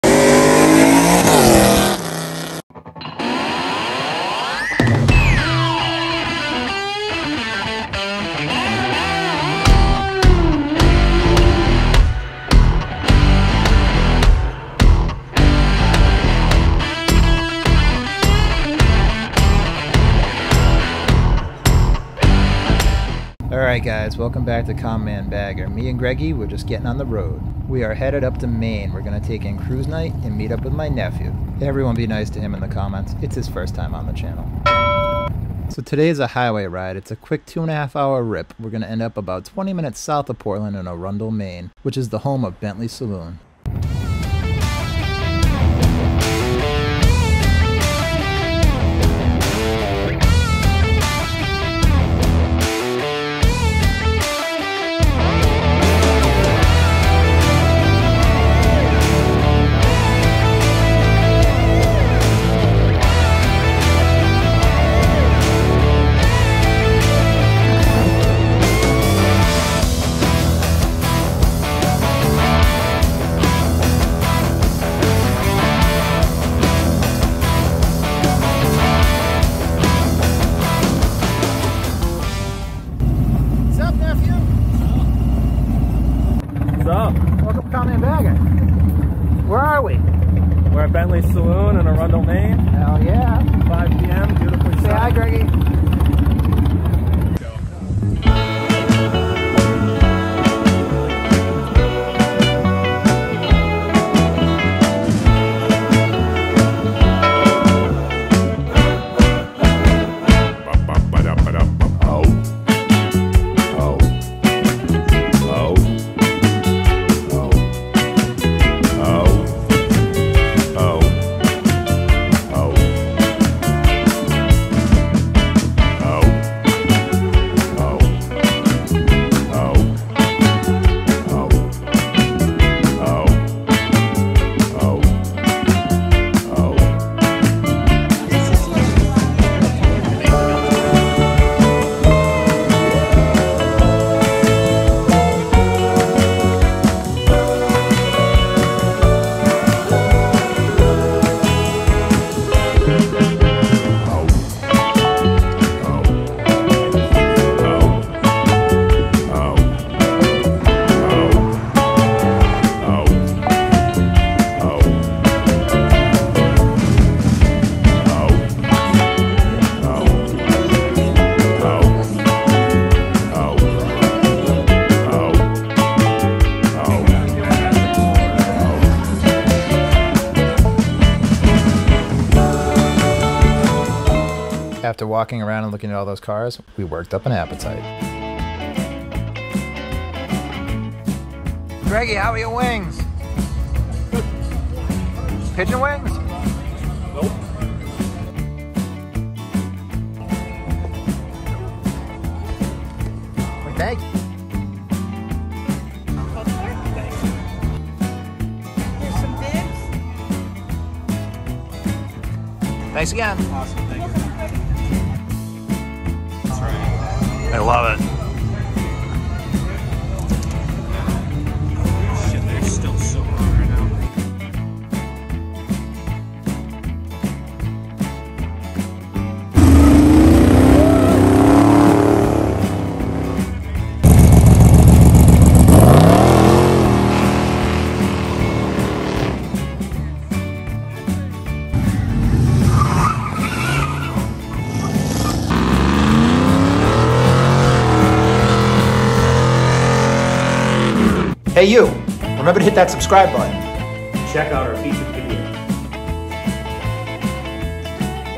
Ride to Bentley's Saloon CRUISE NIGHT Arundel Maine
Common Man Bagger takes a ride to Arundel Maine to take in Cruise Night at Bentley's Saloon. We checked out all of the classic cars and worked up an appetite. It wouldn't be a trip to Maine without Lobsters and BURNOUTS!!  Also it is Andrew's first appearance on the channel.